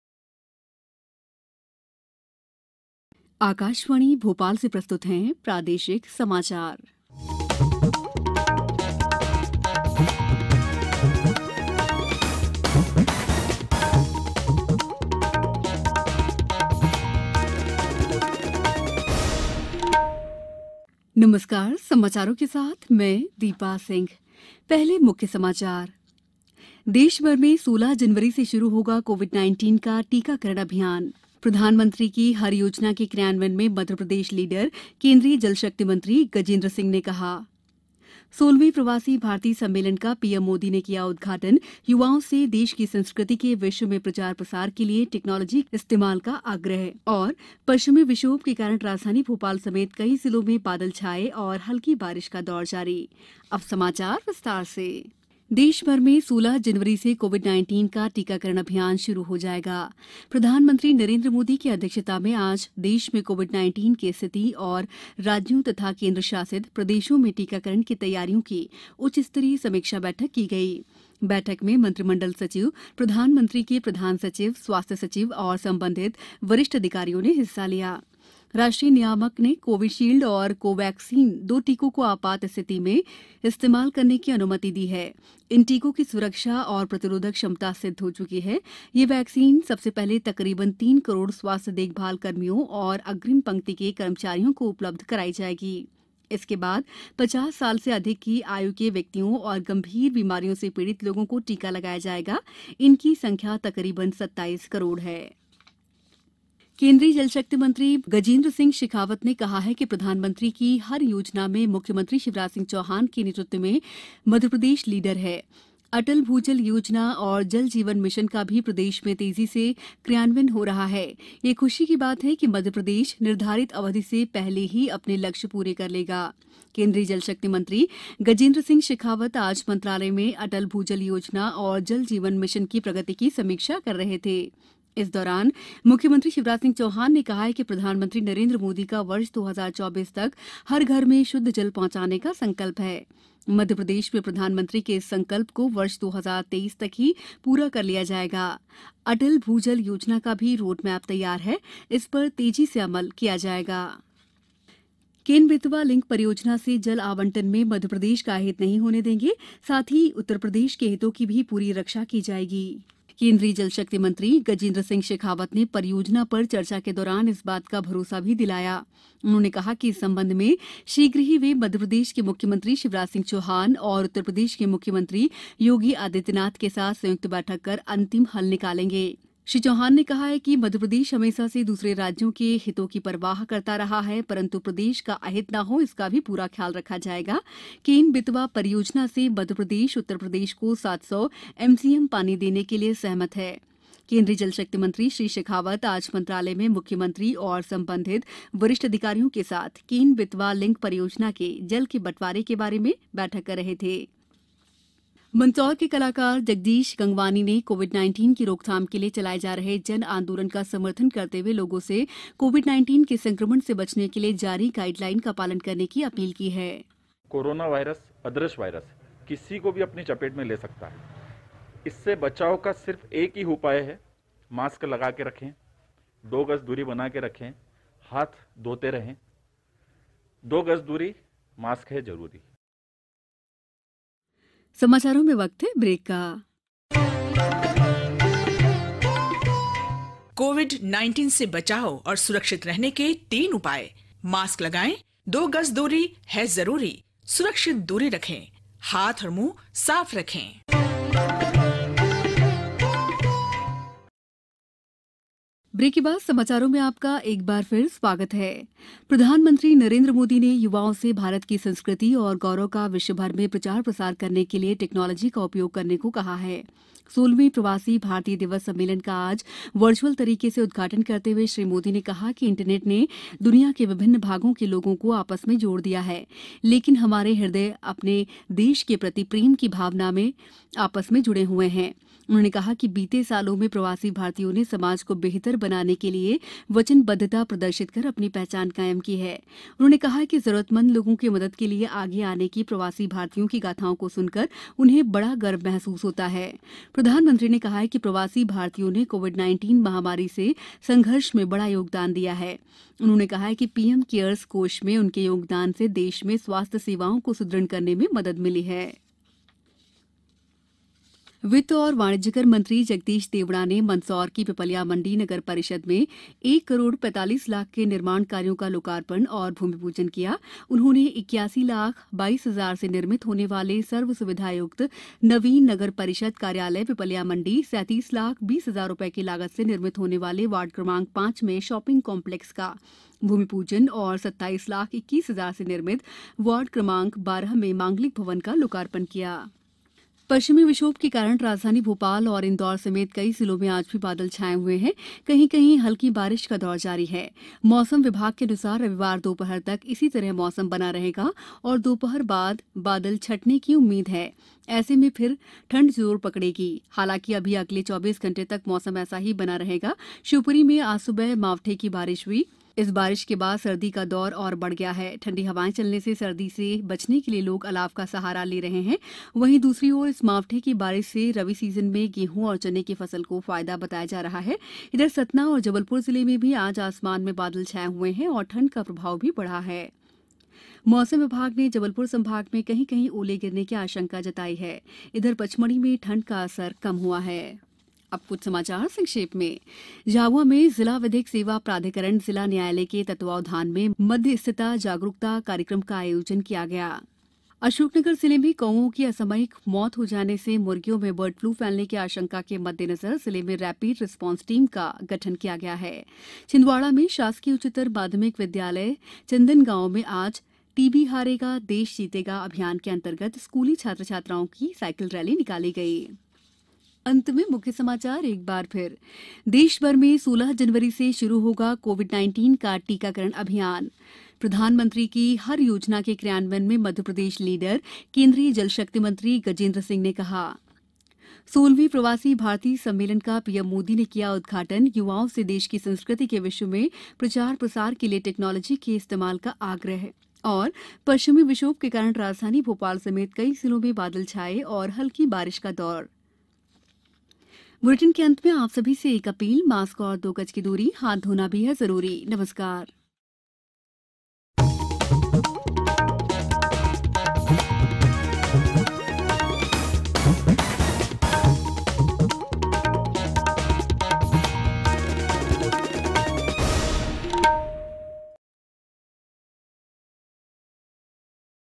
0901 EVENING NEWS BULLETIN
देशभर में 16 जनवरी से कोविड-19 का टीकाकरण अभियान शुरू हो जाएगा।